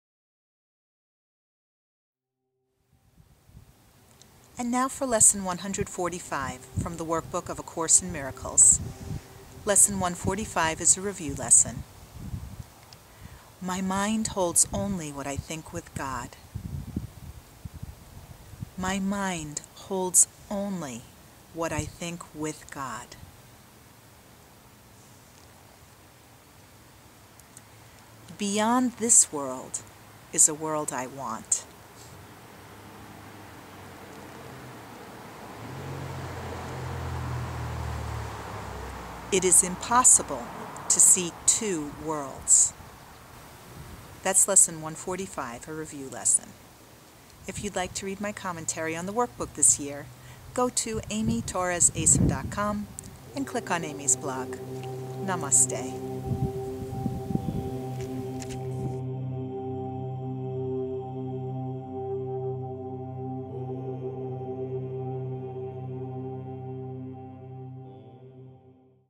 Workbook Lesson 145 from A Course in Miracles with Amy Torres
Lesson 145:  My mind holds only what I think with God.
This reviews Lessons 129 & 130.